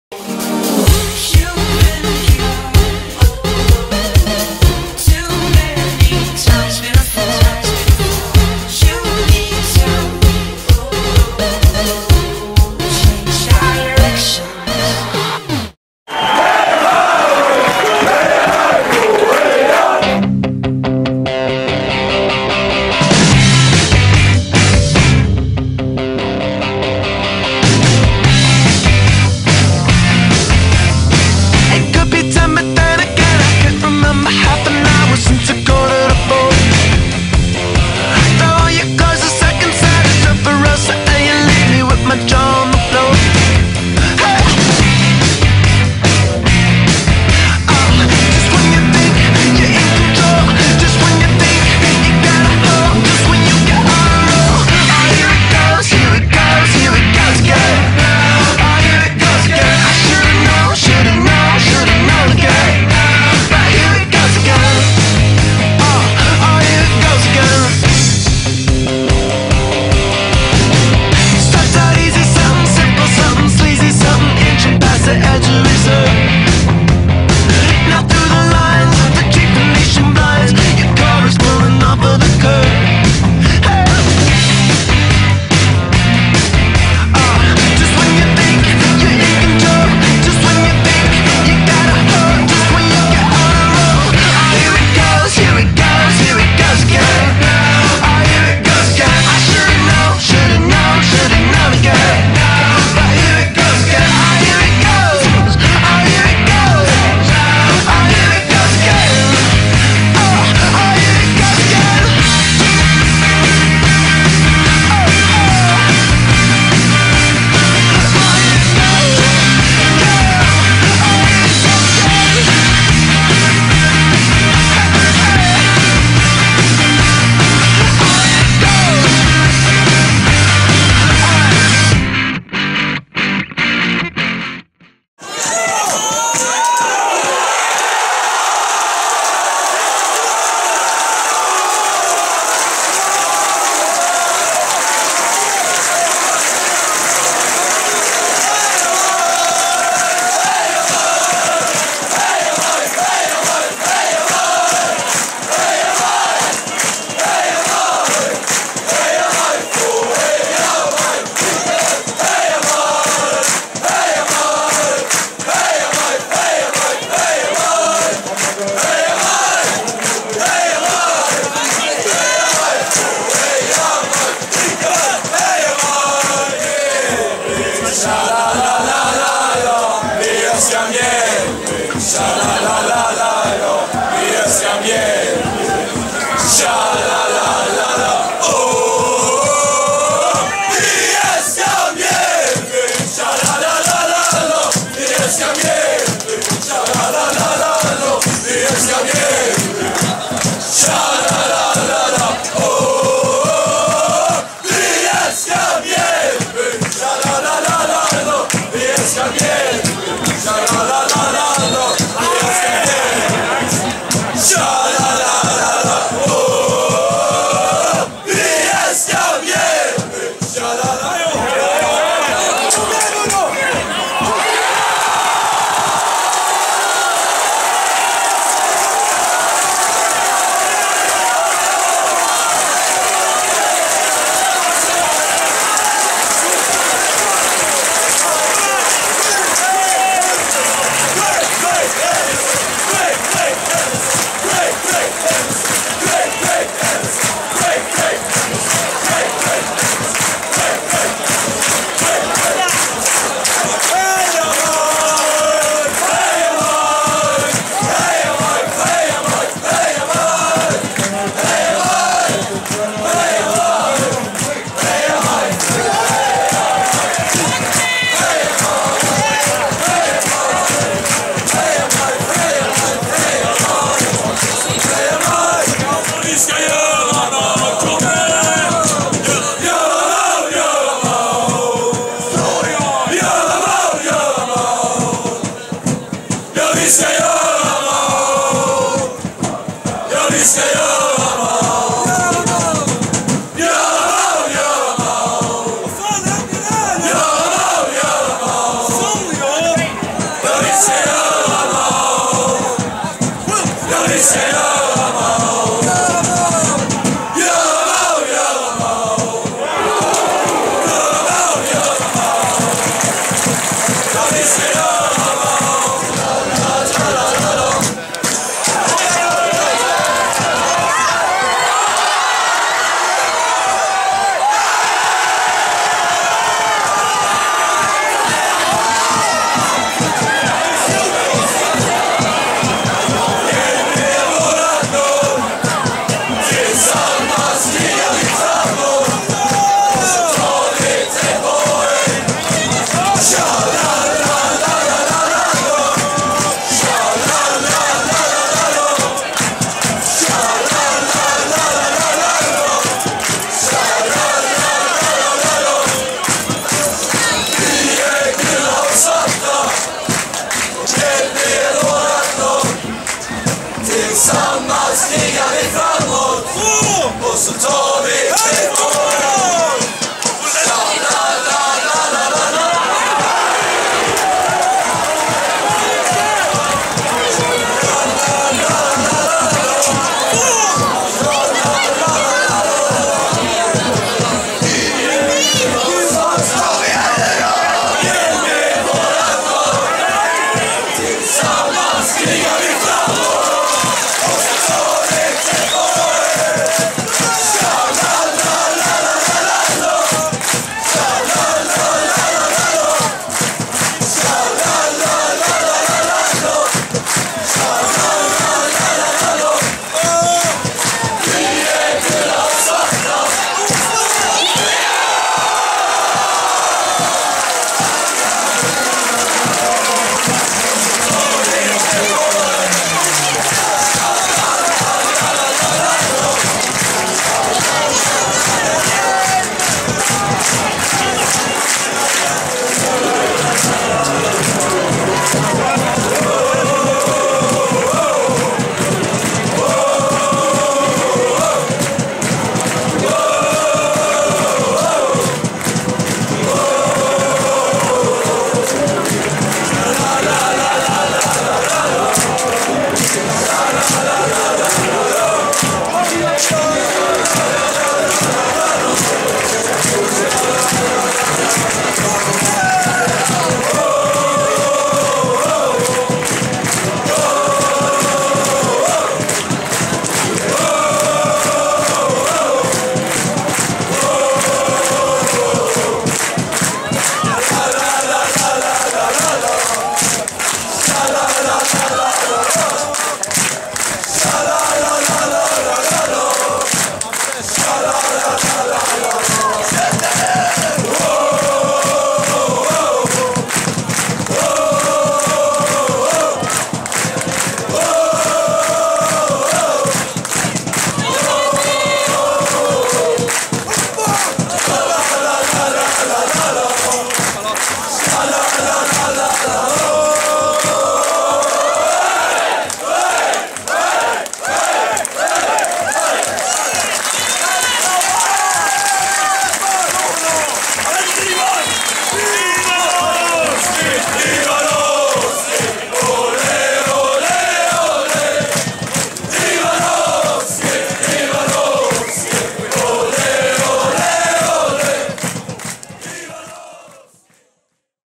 Sillastrybarna produktion: MAIF - IFK GBG
Film ifrån Mjällby AIF - IFK Göteborg. Slutresultat 2 - 1.